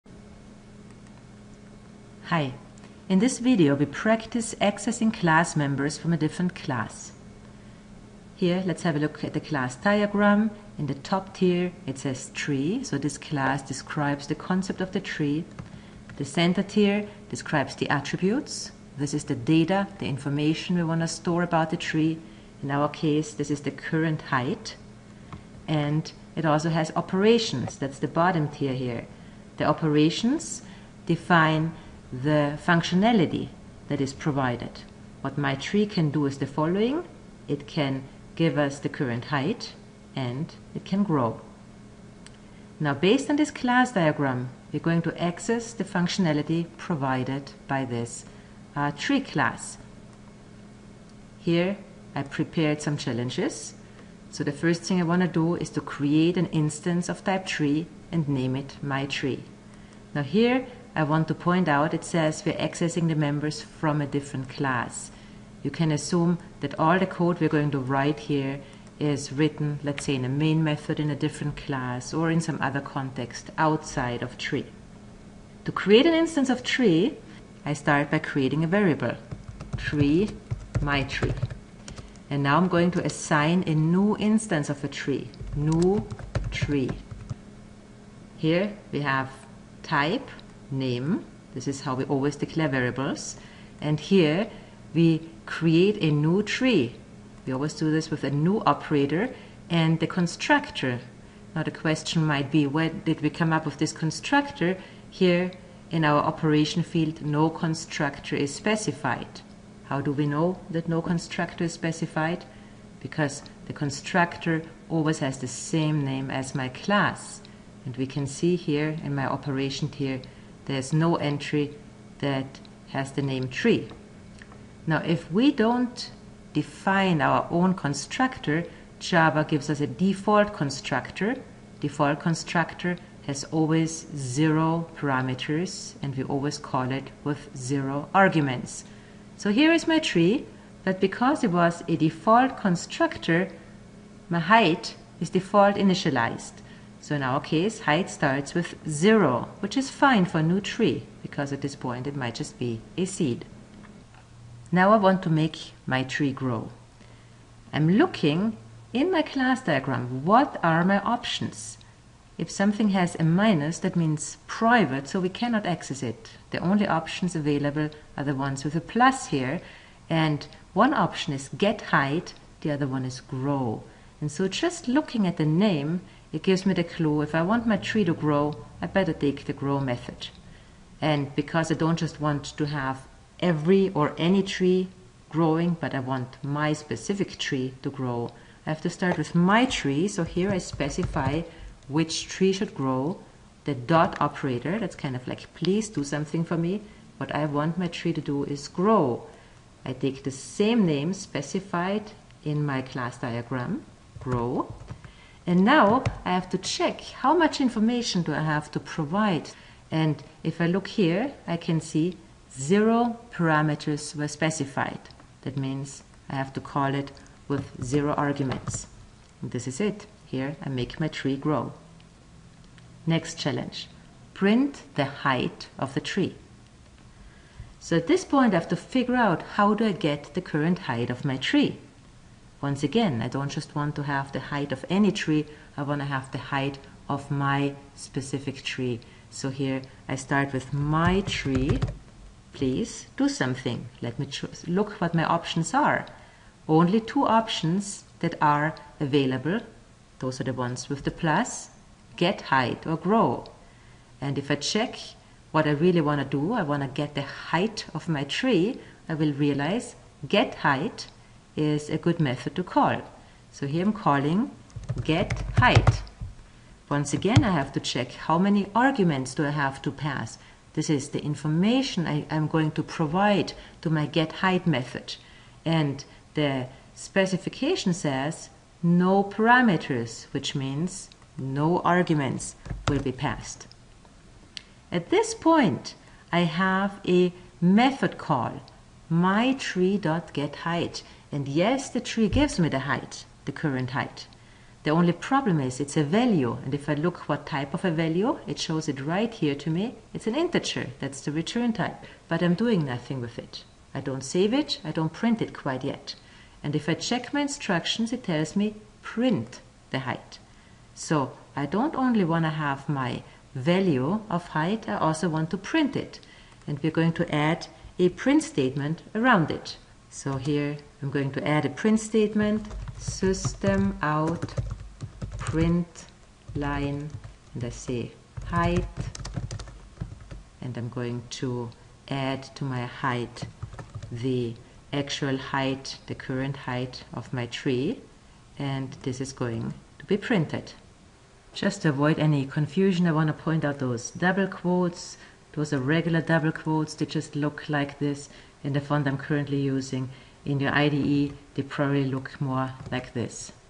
Intro to Classes - accessing fields with getters
This video walks you through a practice exercise to help you understand how to create an instance (object) of a class and how to access methods from another class.
Based on a UML diagram of a simple class Tree it shows you how to create a tree object, how to access the field height with a getter, and how to make the tree grow by calling a method.